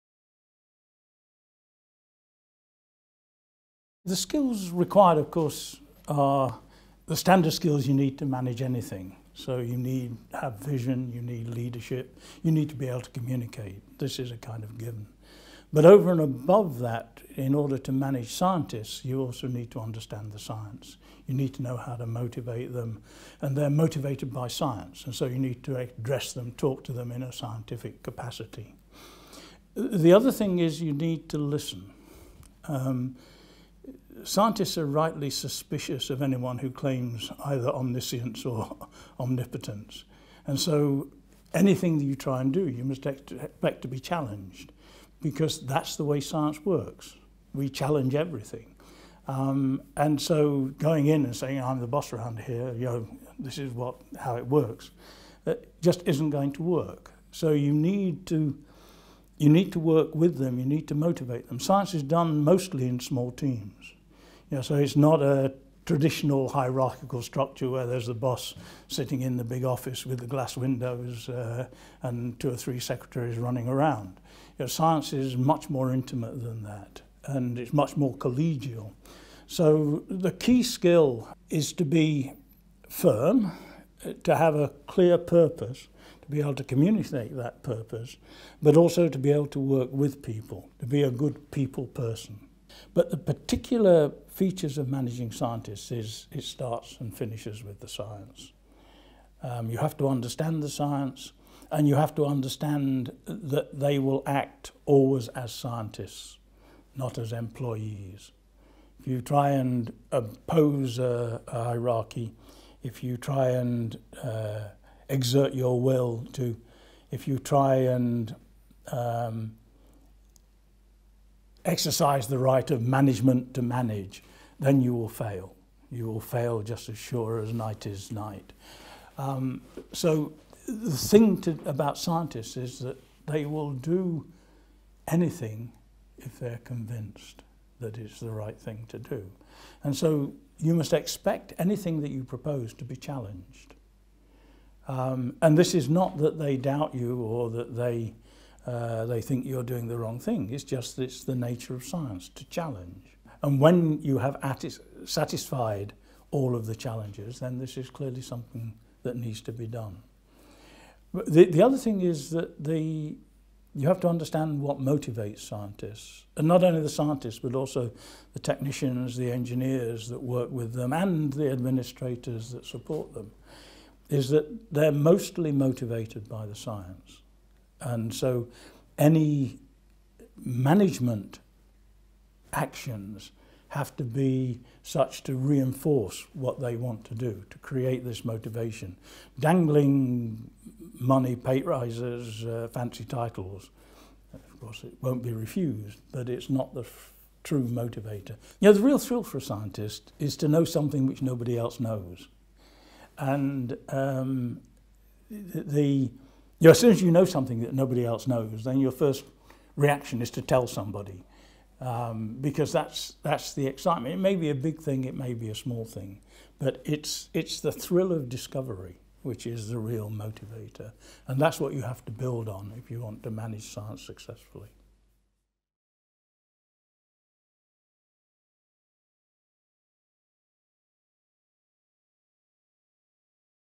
The Most Important Skill When Learning to Manage Scientific Research
Ken Peach discusses what's important when learning to manage scientific research.